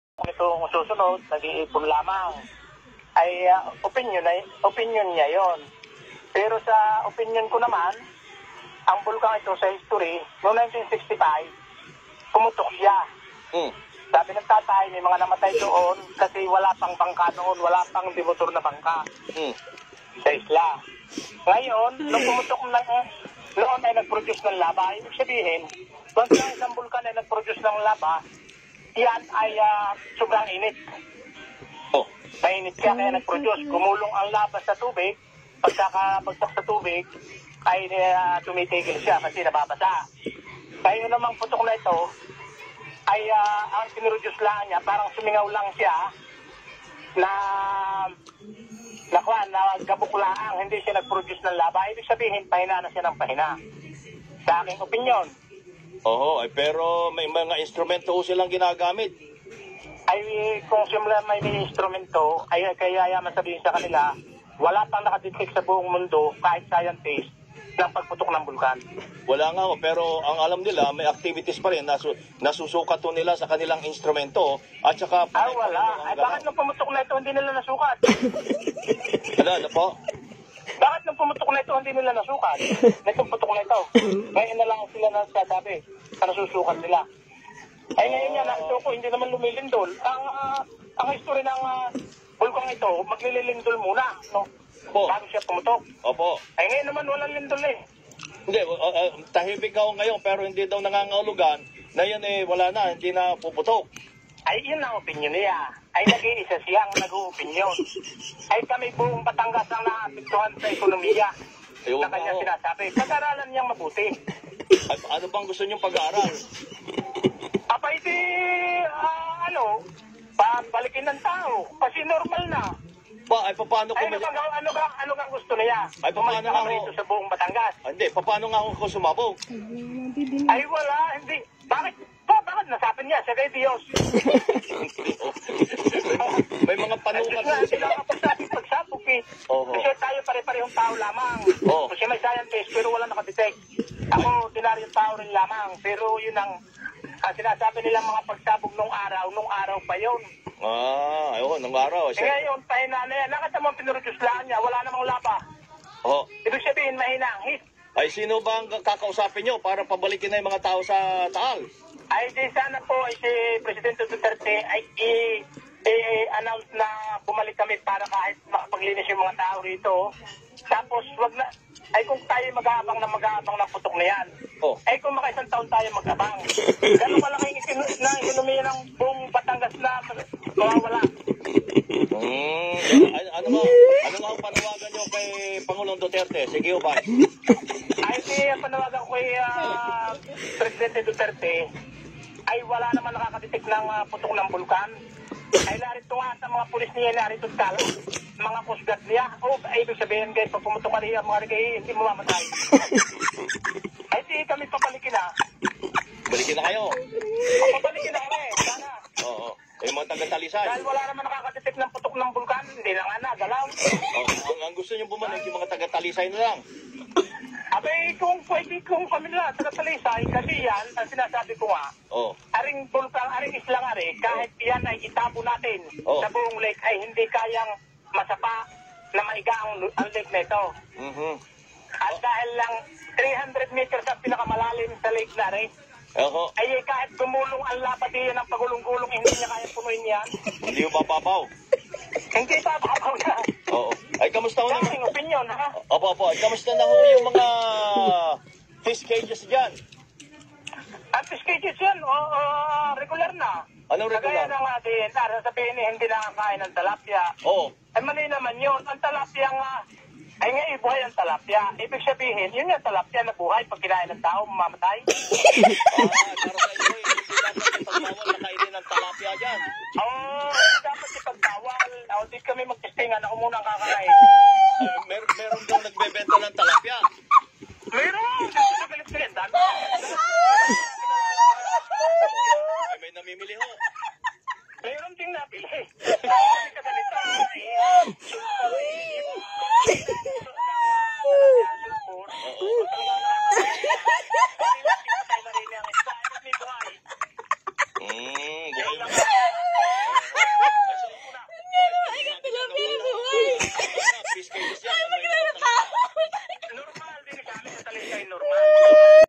Talisay Vice Mayor to Phivolcs: Siya ga ay Diyos | LAPTRIP
Panoorin ang interview kay Vice Mayor Charlie Natanauan Ng Talisay, Batangas. Gusto nya kasi pabalikin na ang mga kababayan sa kani-kanilang mga bahay sa kabila Ng pagtutol Ng Phivolcs.